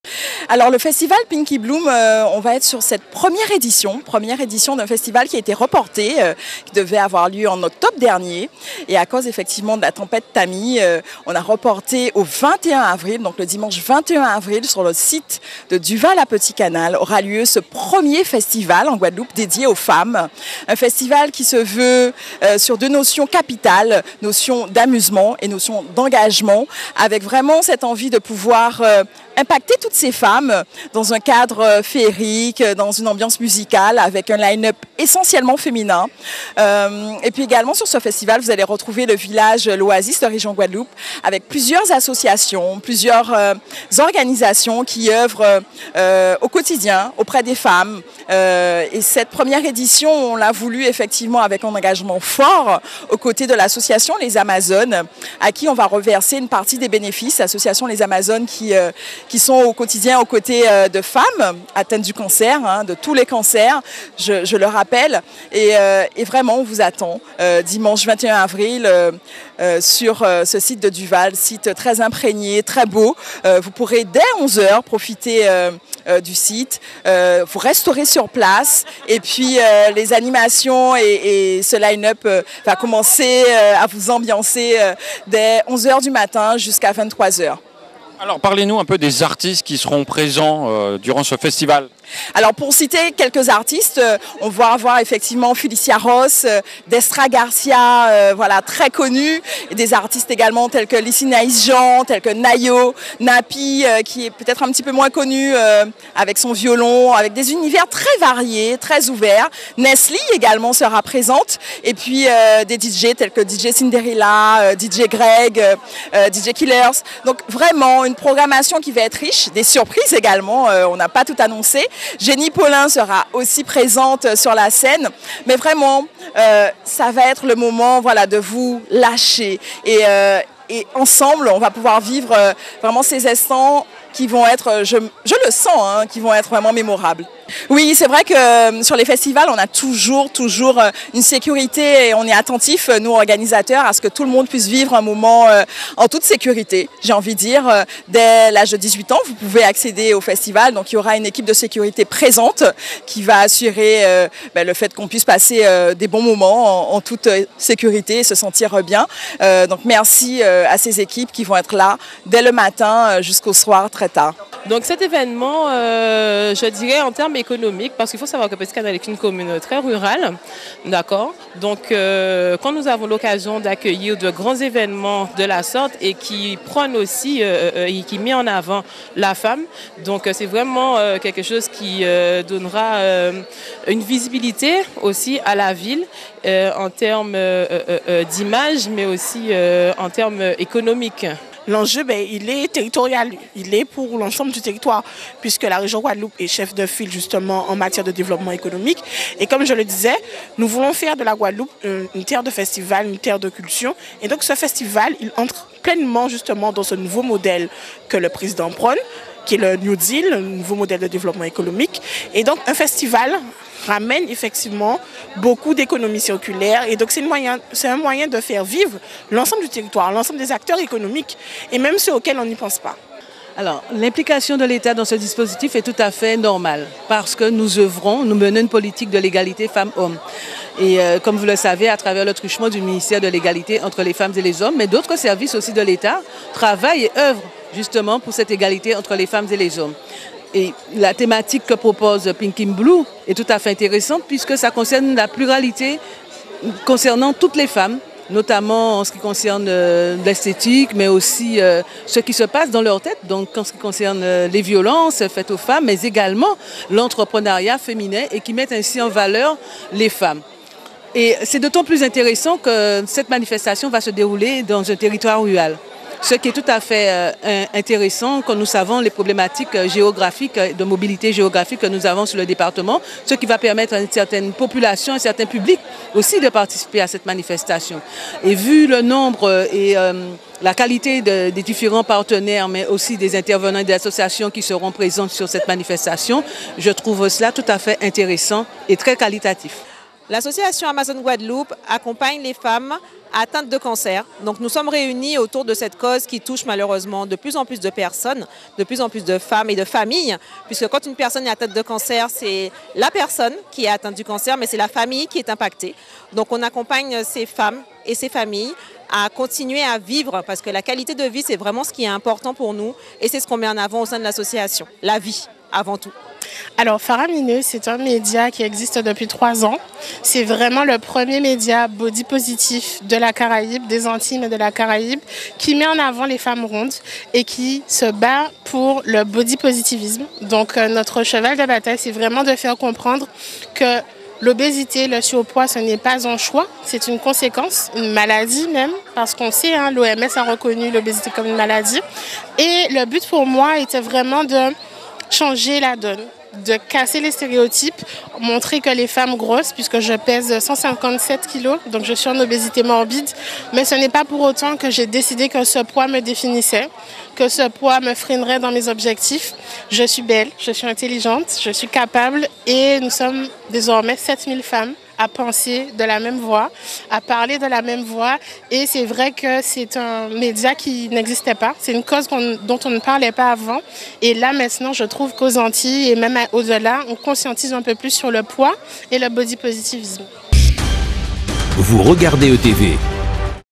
Pinky Bloom: Un Festival en Guadeloupe célèbre les femmes et leur créativité
PETIT CANAL, GUADELOUPE — Ce dimanche 21 avril, le site de Duval à Petit Canal sera animé d'une effervescence particulière avec la première édition du festival Pinky Bloom, un événement dédié à la célébration des femmes.